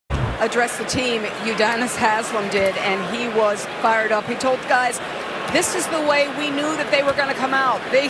Udonis Haslem addressed the team during the last timeout. 💯
Boston Celtics vs Miami Heat - Game 4